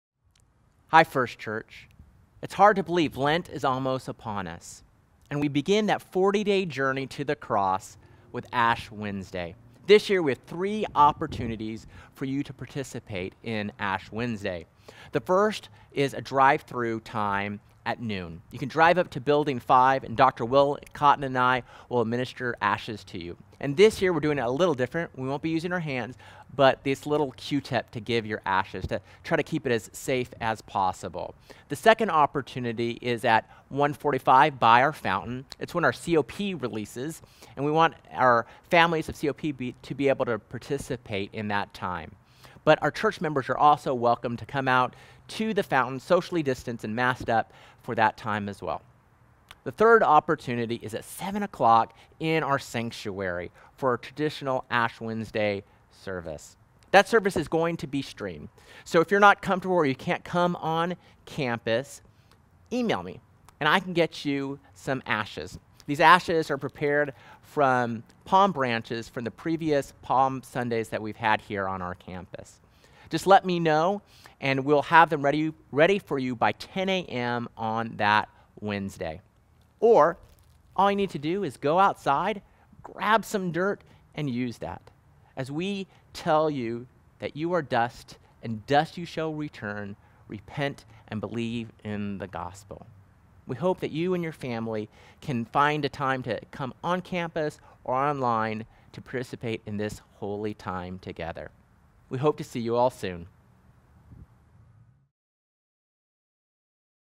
Ash Wednesday 2021 Services Schedule
Ash Wednesday Services will be:
12:00 - at the circle drive in front of the main office building
1:45 - at the Fountain on campus
7:00 - in our sanctuary, this service will be streamed on our website